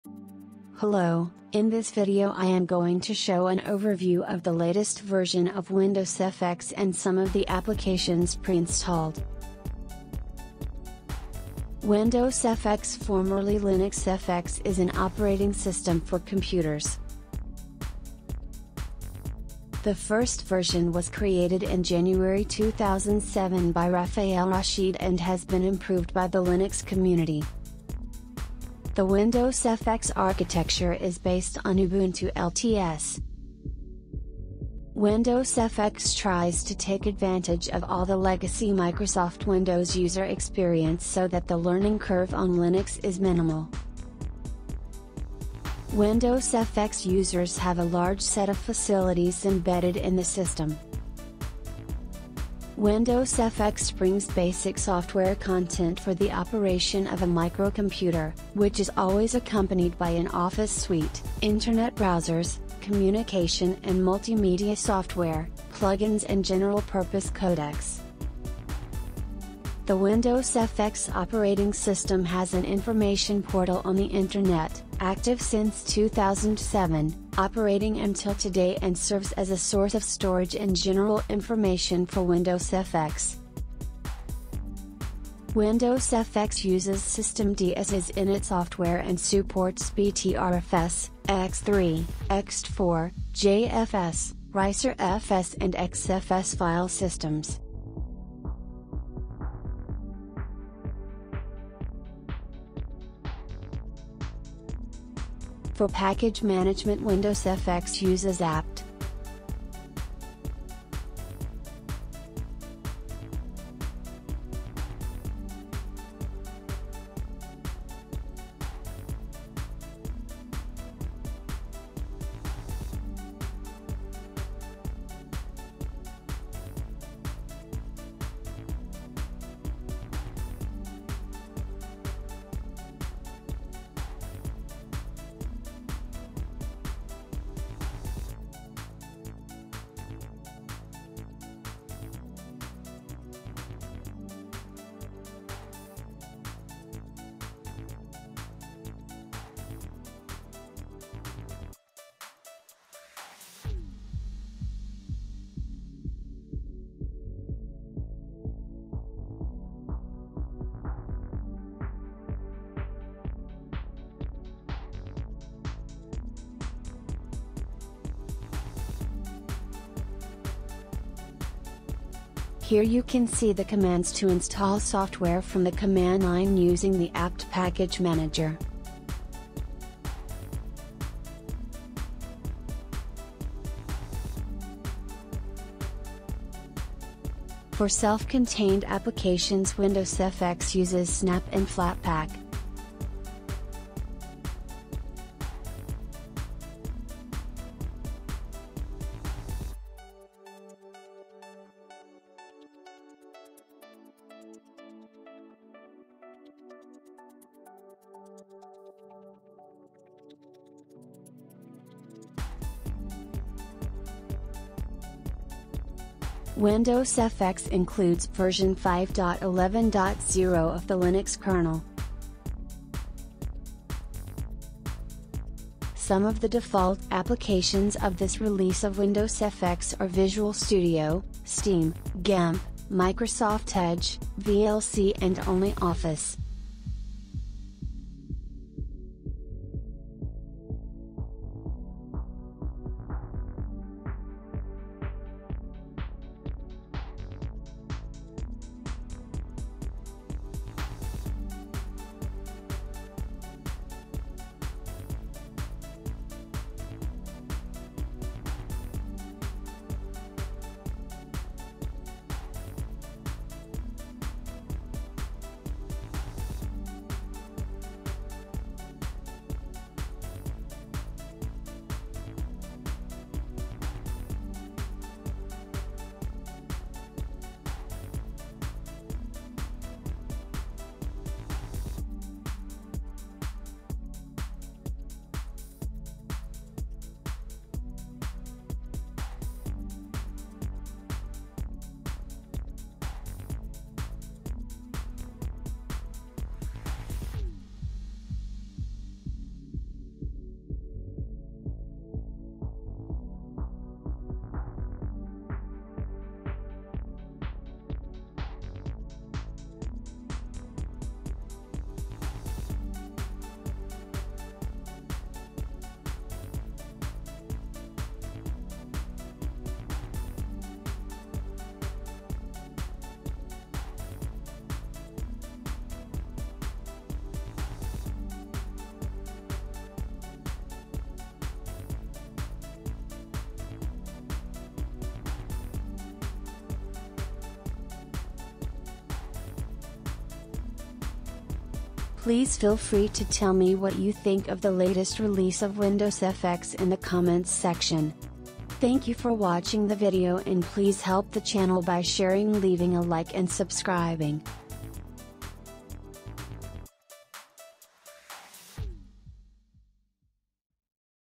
Linux overview | Windowsfx 11 Preview Free Edition
In this video, I am going to show an overview of Windowsfx 11 Preview Free Edition and some of the applications pre-installed.

Linuxfx / Windowsfx  is a Brazilian Linux distribution based on Ubuntu. It ships with an intuitive KDE desktop user interface designed to facilitate migration of users from Windows.
The distribution include a new personal assistant, a WX theme for desktop and system applications, and compatibility with software written for Windows (.exe and .msi) through a Wine port. Following the release of Linuxfx 10.6 the distribution became a commercial offering.